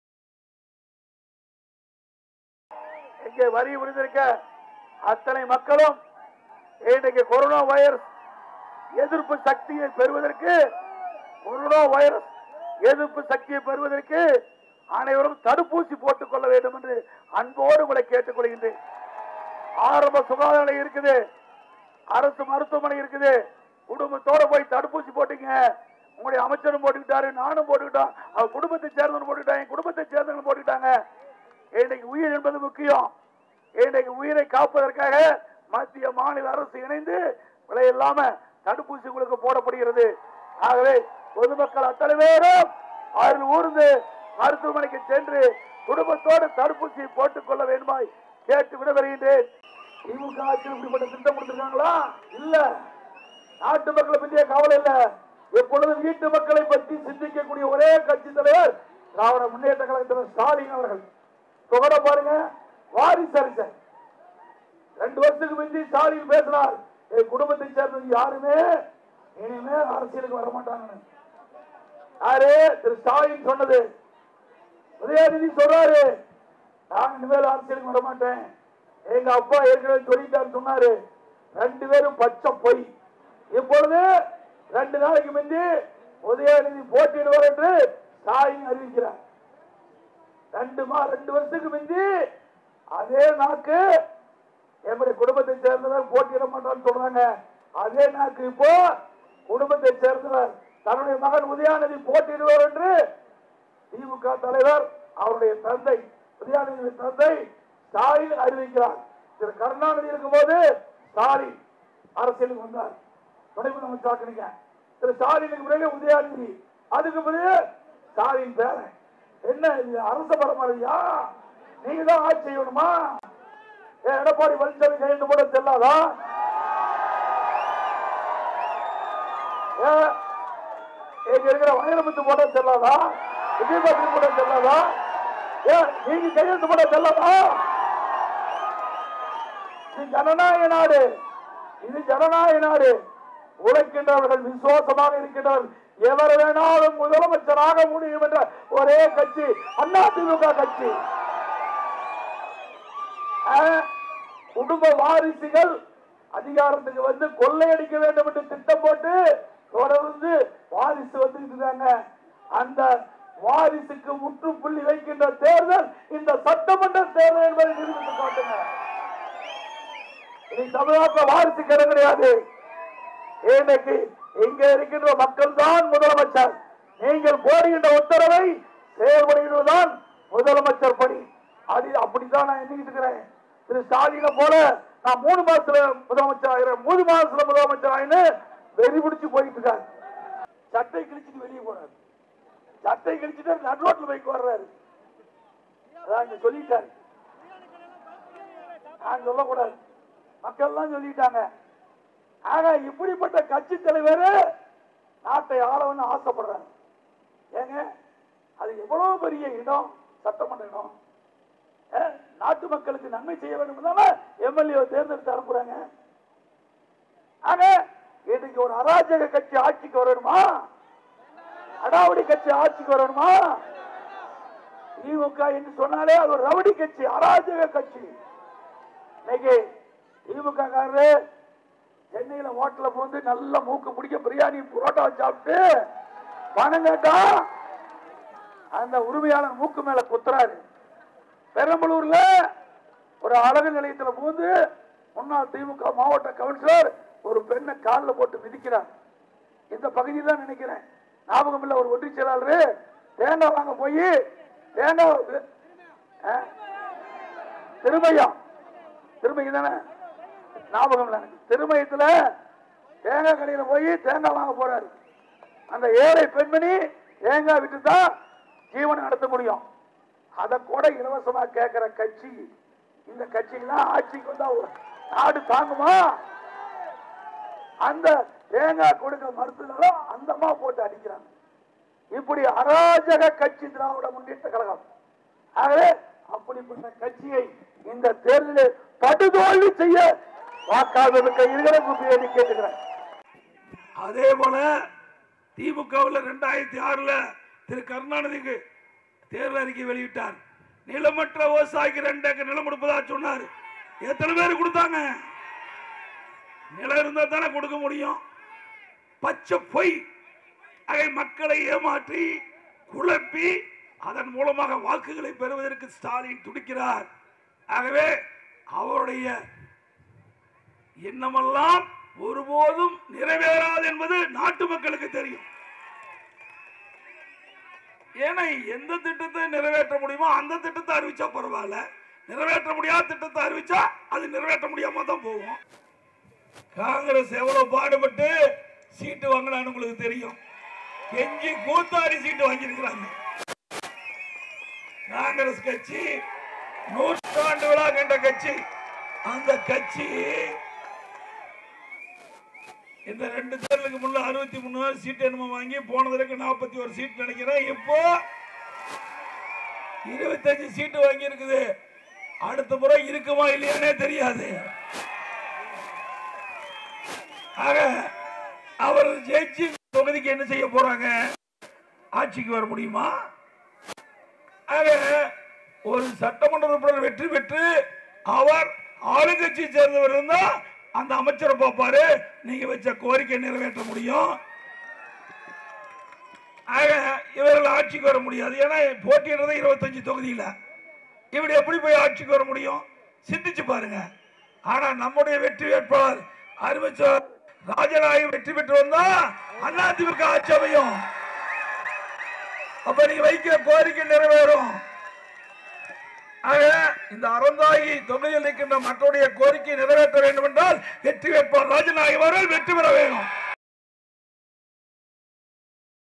குடும்பத்துடன் CORONA தடுப்பூசி போட்டுக் கொள்ளுங்கள் - முதல்வர் எடப்பாடி K.பழனிசாமி வேண்டுகோள்.!!
குடும்பத்துடன் கொரானா தடுப்பூசி போட்டுக் கொள்ளுங்கள் - முதல்வர் எடப்பாடி கே. பழனிசாமி வேண்டுகோள்.!!